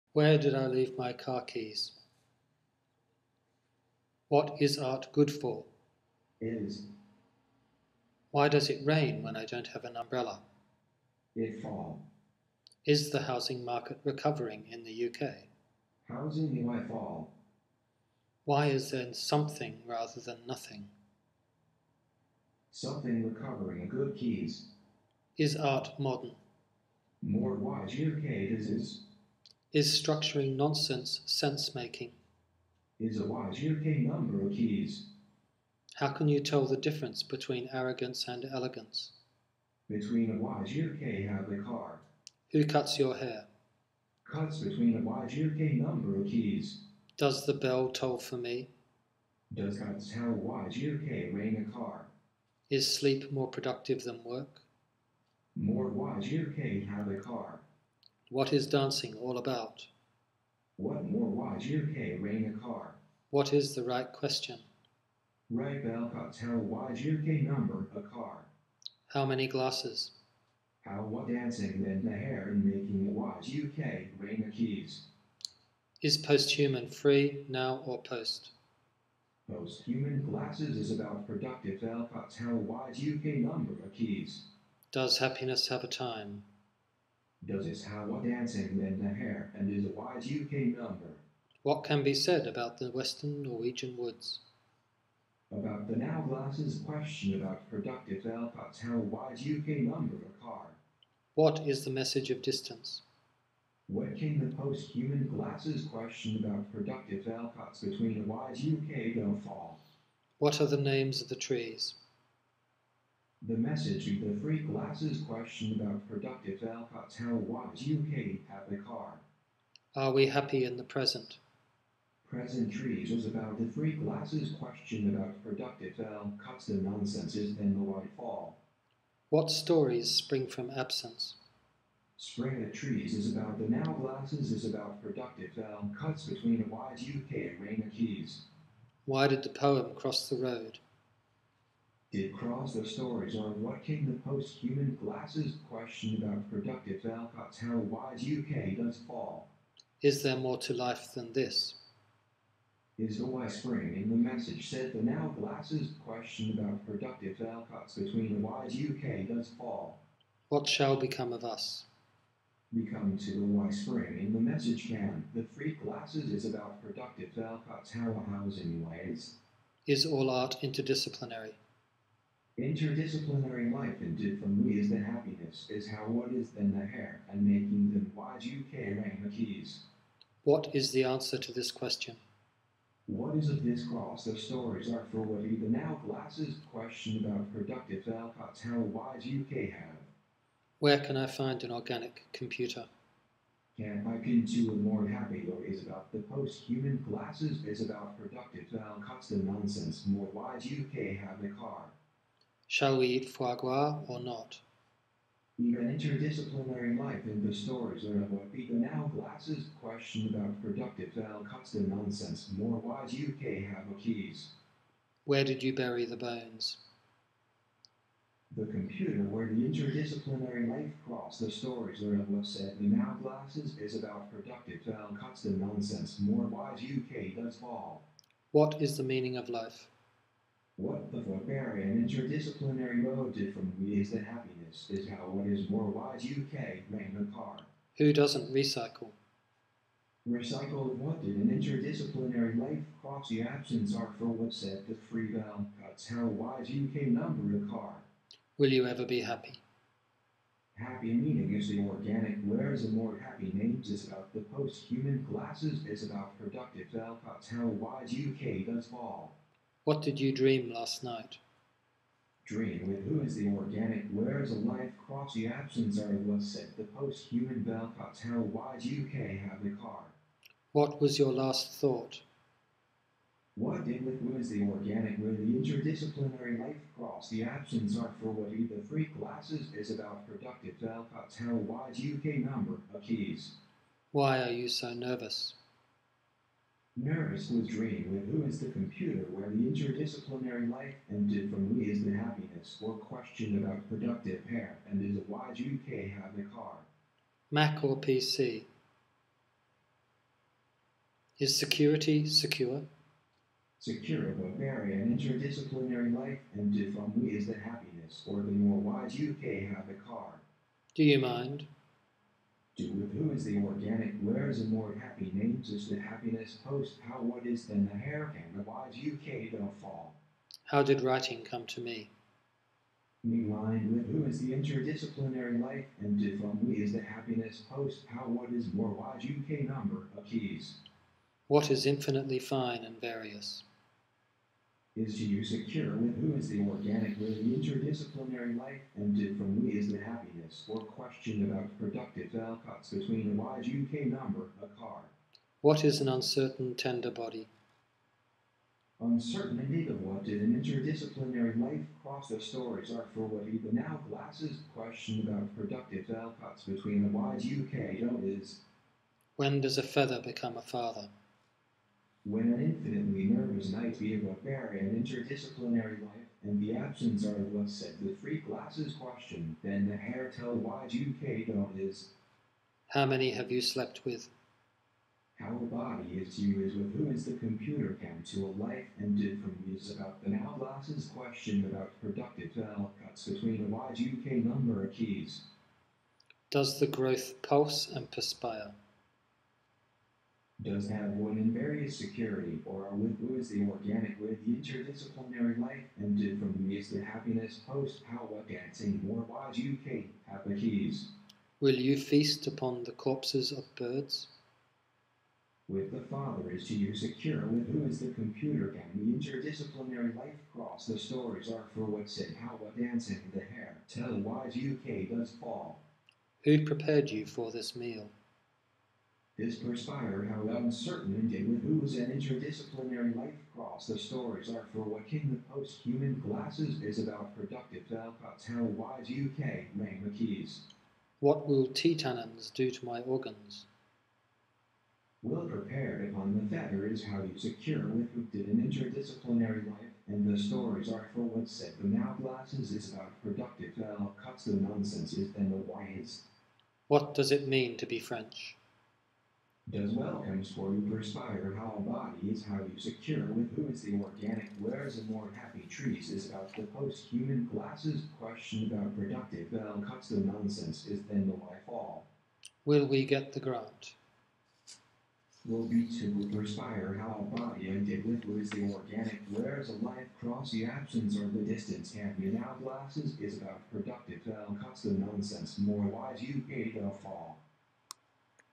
Oracle (2009)
screen documentation of interactive performance text work. by Simon Biggs